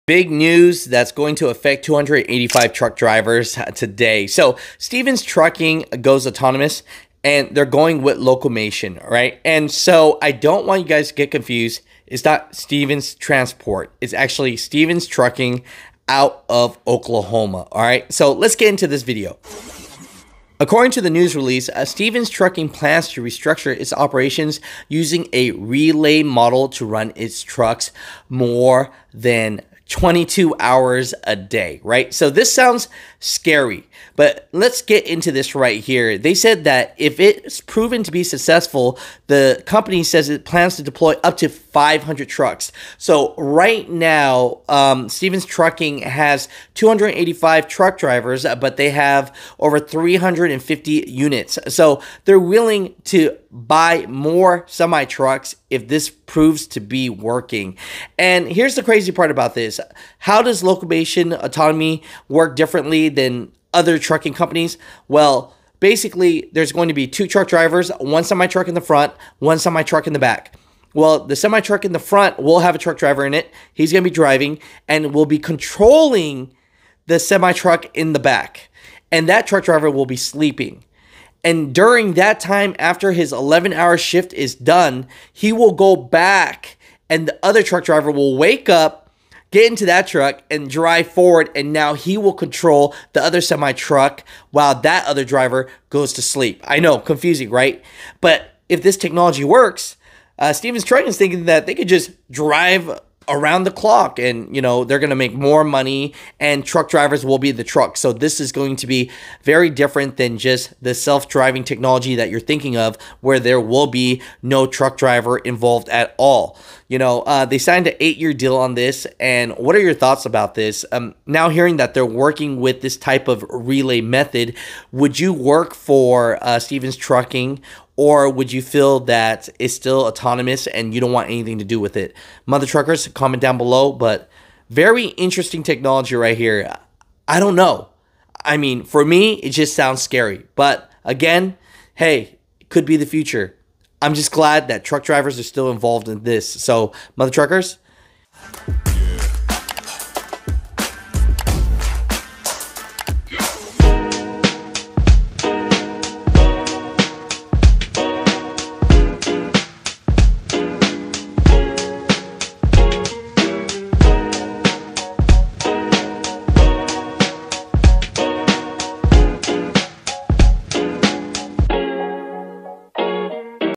285 Truck Drivers Just Found Out That Their Company Is Going Self Driving 🤯
285 Truck Drivers Just Found Out That Their Company Is Going Self Driving 🤯